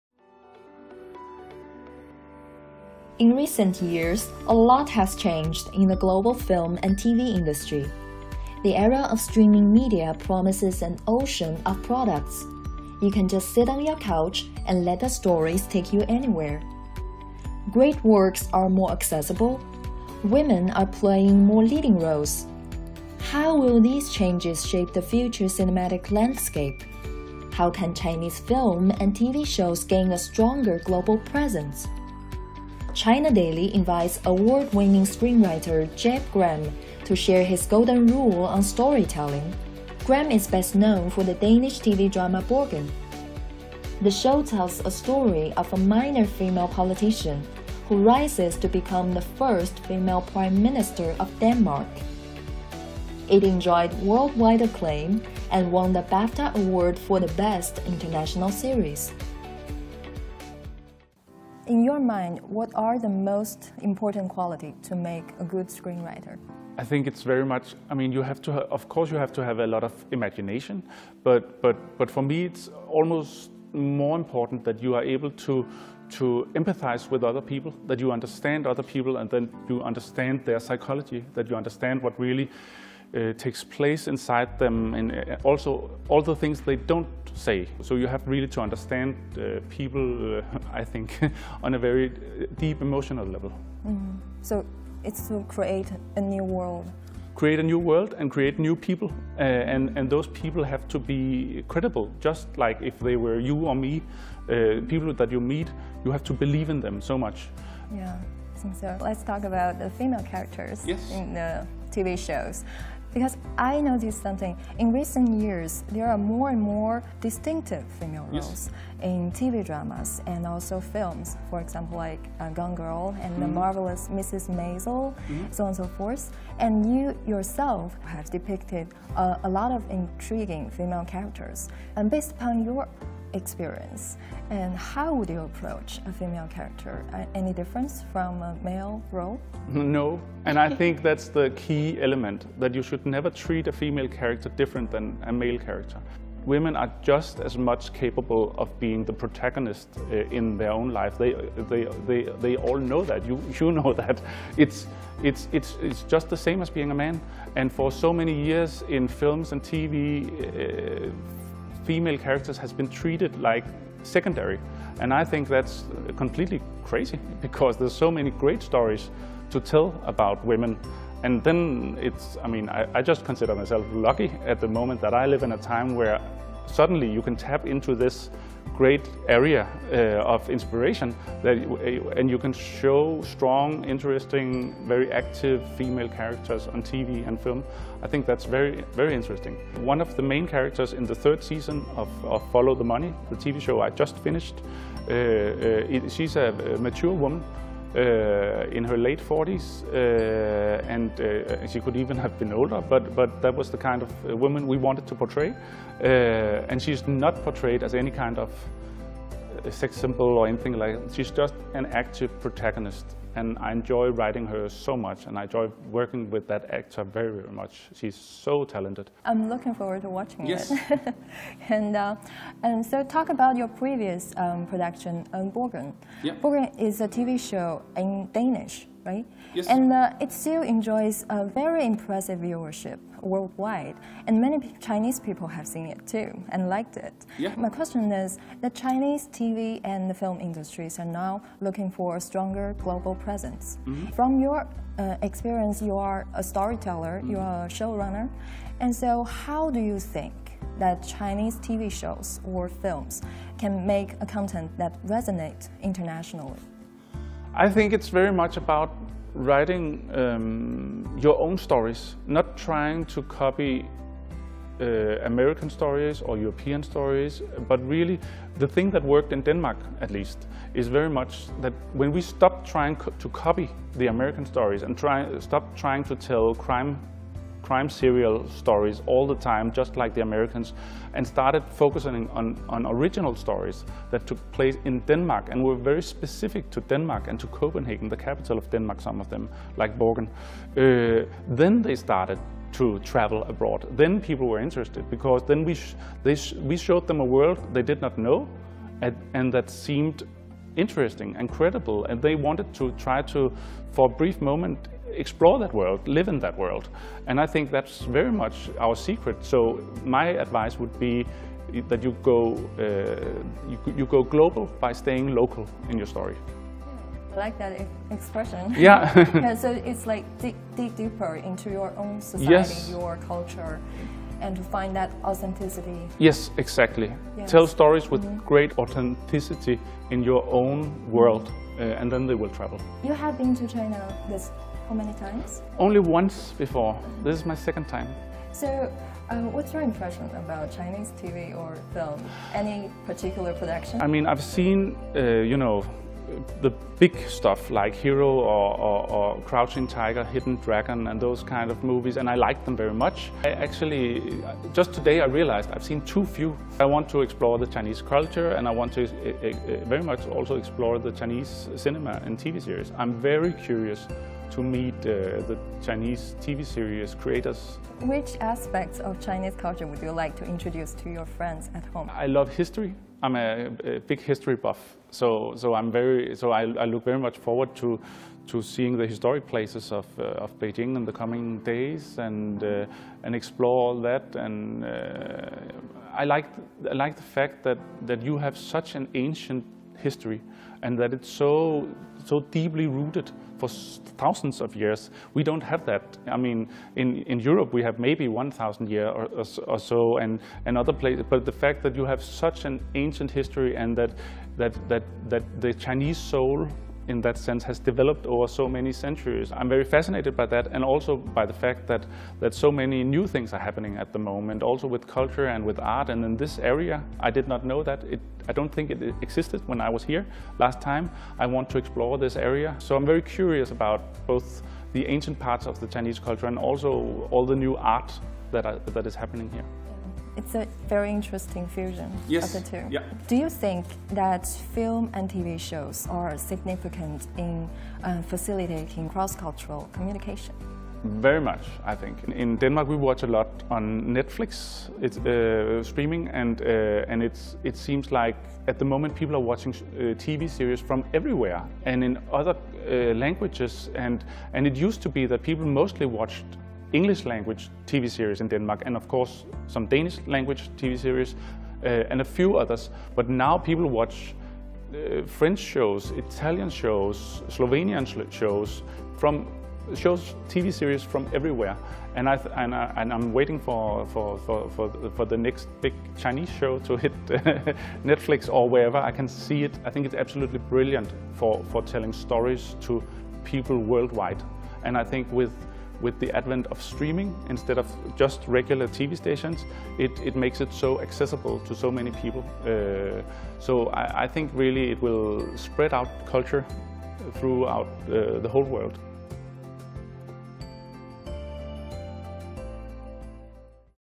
Going global by staying local: Danish screenwriter Jeppe Gram's golden rule
In recent years, a lot has changed in the global film and TV industry. The era of streaming media promises an ocean of products. You can just sit on your couch and let the stories take you anywhere. Great works are more accessible.

Women are playing more leading roles. How will these changes shape the future cinematic landscape? How can Chinese films and TV shows gain a stronger global presence? China Daily invites award-winning screenwriter JeppeGram to share his golden rule on storytelling.

Gram is best known for the Danish TV drama Borgen. The show tells the story of a minor female politician who rises to become the first female prime minister of Denmark. It enjoyed worldwide acclaim and won the BAFTA award for best international series. (Reporter: Li Wenrui)